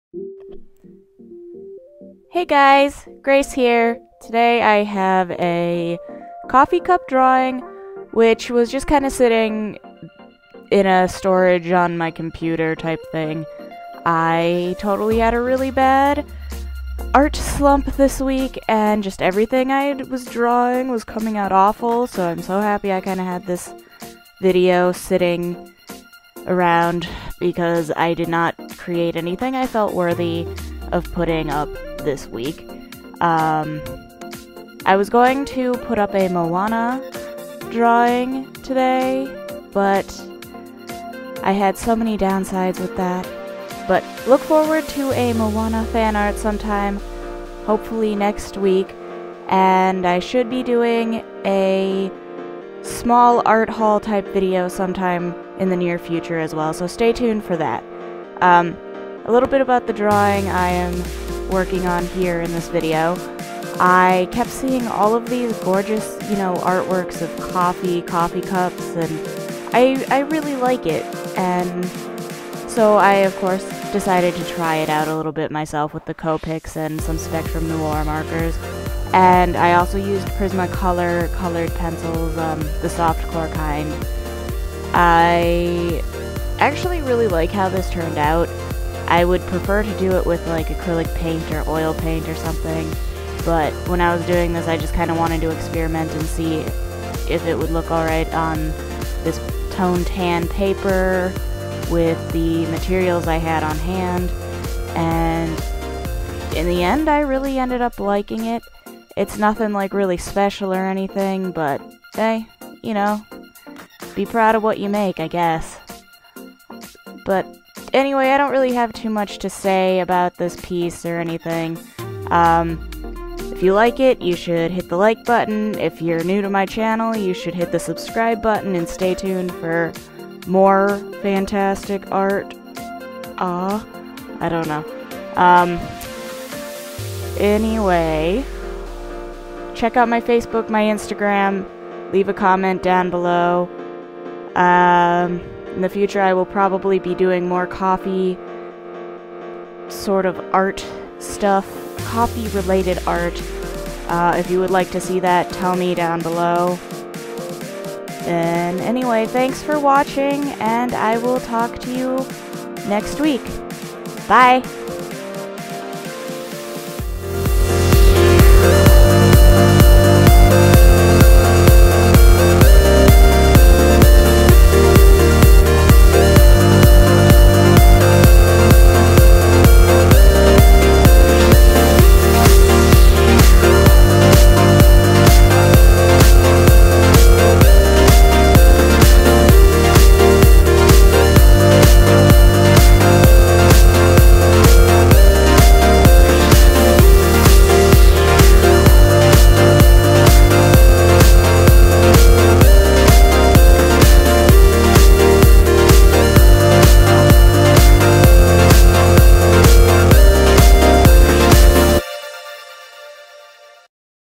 Just Some Coffee
Drawing and coloring a cup full of coffee.

Materials: Strathmore toned tan paper
Copic markers
Spectrum noir markers
Prismacolor colored pencils
Copic multiliner